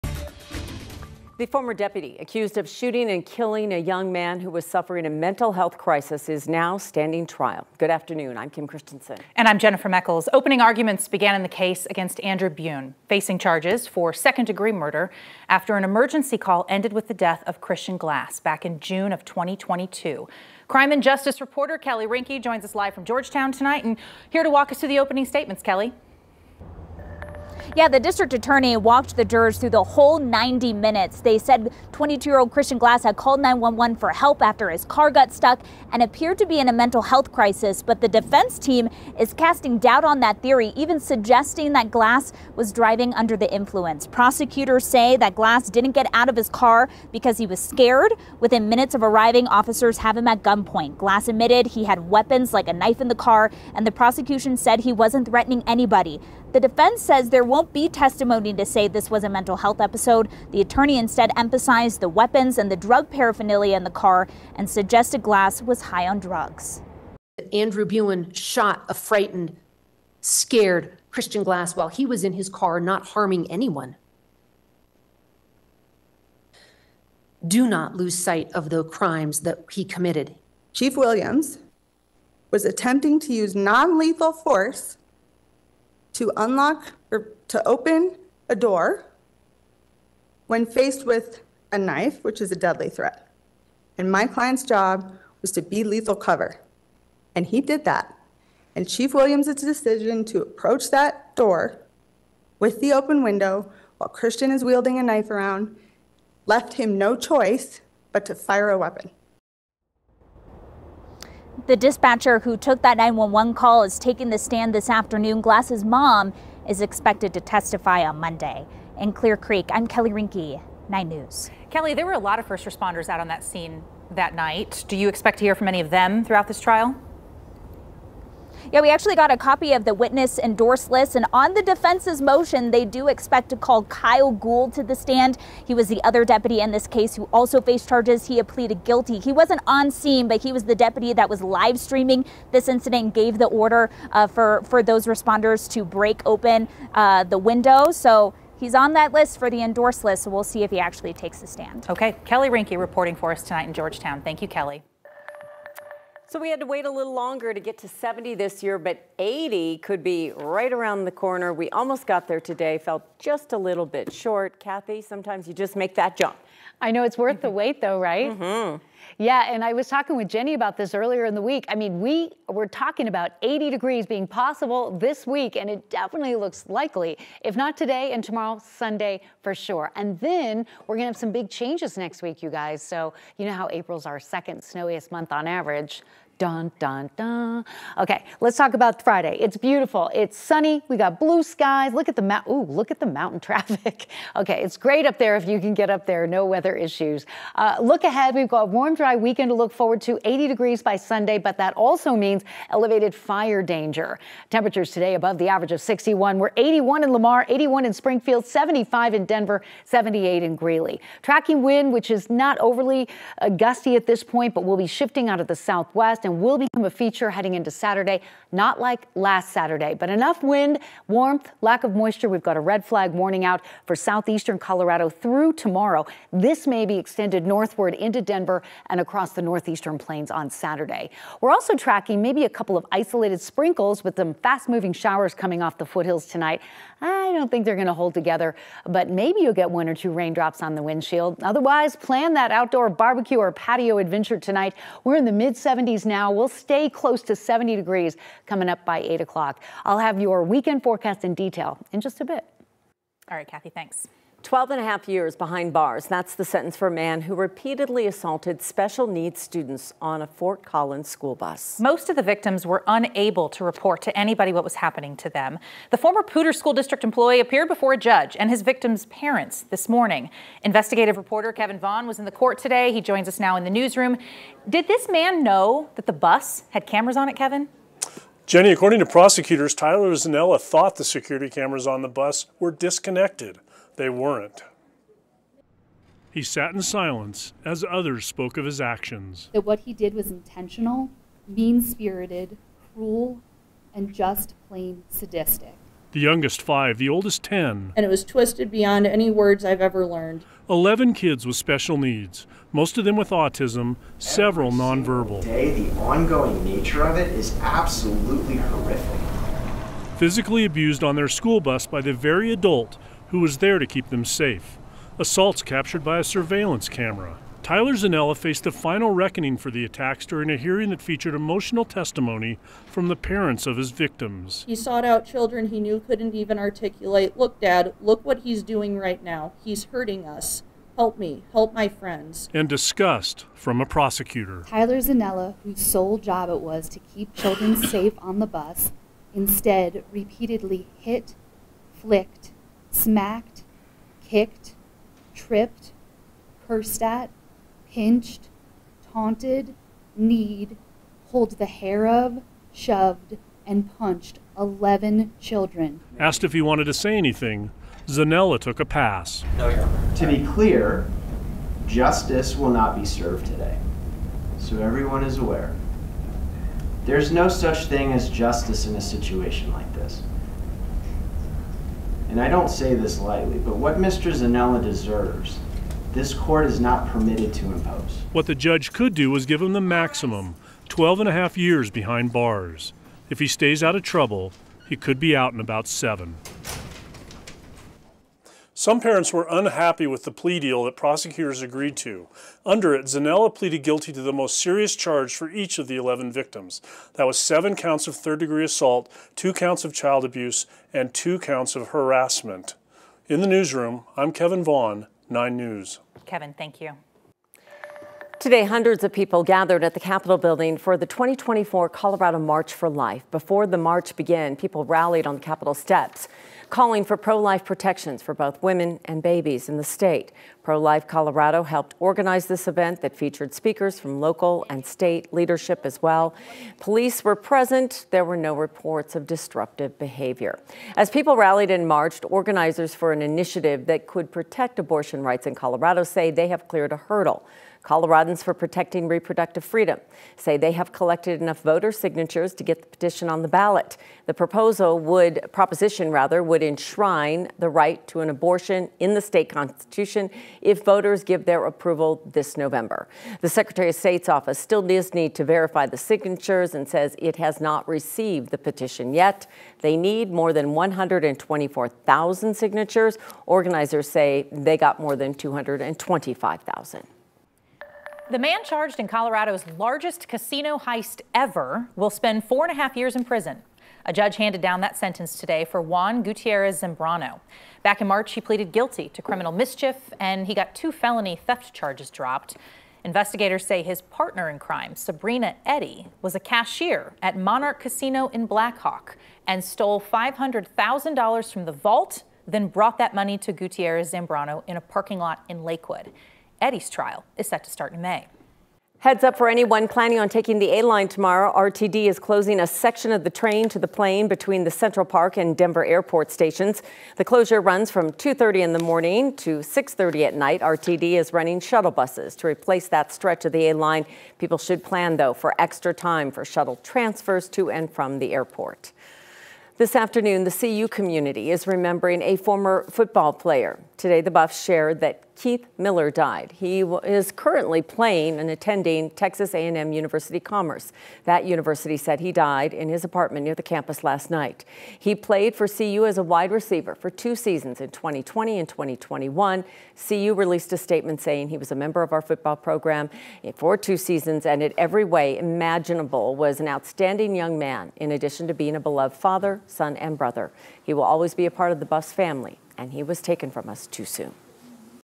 Latest headlines | Trial opens for former deputy charged in death of Christian Glass
Here are the top stories from the 4 p.m. 9NEWS newscast on Friday, April 12.

9NEWS (KUSA) is located in Denver, Colorado.